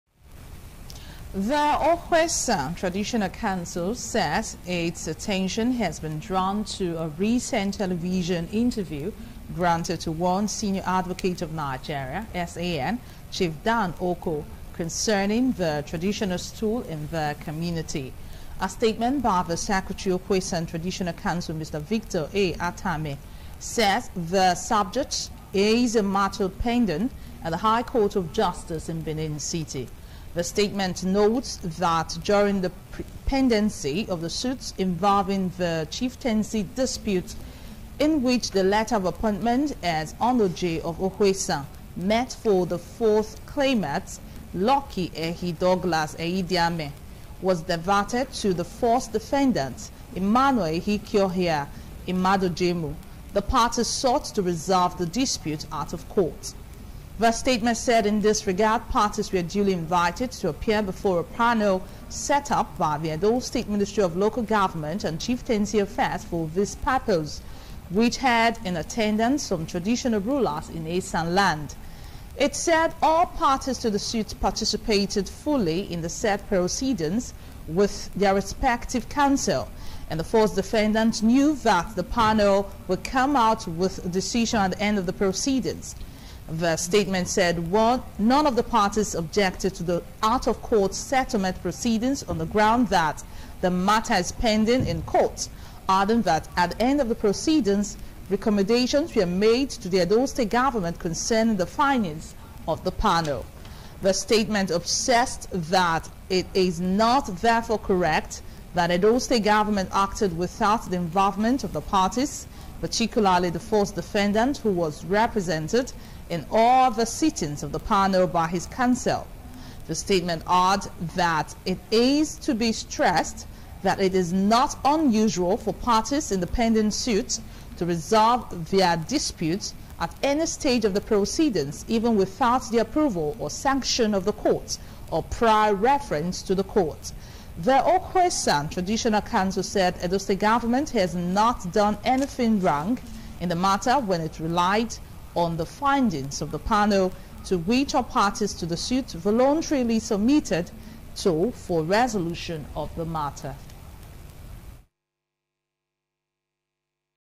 Okhuesan Traditional Council reacts to Television Interview granted to Chief Dan Okoh
The Okhuesan Traditional Council says its attention has been drawn to a recent Television interview granted to one Senior Advocate of Nigeria, SAN, Chief Dan Okoh concerning the Traditional Stool in the Community.

A statement by Secretary Okhuesan Traditional Council, Mr. Victor A. Ataime says the subject is a matter pending at the High Court of Justice, Benin City.

The Statement notes that during the pendency of the suit involving the Chieftaincy dispute, in which the letter of appointment as Onogie of Okhuesan meant for the 4h Claimant Lucky Ehi-Douglas Ehidiamen, was diverted to the First Defendant, Emmanuel Ehikioya Imadojemu, the parties sought to resolve the dispute out of court.

The Statement said in this regard, parties were duly invited to appear before a panel set up by the Edo State Ministry of Local Government and Chieftaincy Affairs for this purpose, which had in attendance some Traditional Rulers in Esan Land.

It said all parties to the suit participated fully in the said proceedings with their respective Counsel and the first Defendant knew that the panel will come out with a decision at the end of the proceedings.

The Statement said none of the parties objected to the out-of-court settlement proceedings on the ground that the matter is pending in Court adding that at the end of the proceedings, recommendations were made to the Edo State Government concerning the findings of the panel.

The Statement obsessed that it is not therefore correct that the Edo State government acted without the involvement of the parties, particularly the first Defendant who was represented in all the sittings of the panel by his Counsel.

The Statement adds that it has to be stressed that it is not unusual for parties in a pending Suit to resolve their dispute at any stage of the proceedings even without the approval or sanction of the Court, or prior reference to the Court.

The Okhuesan Traditional Council said Edo State Government has not done anything wrong in the matter when it relied on the findings of the panel to which all parties to the suit voluntarily submitted for resolution of the matter.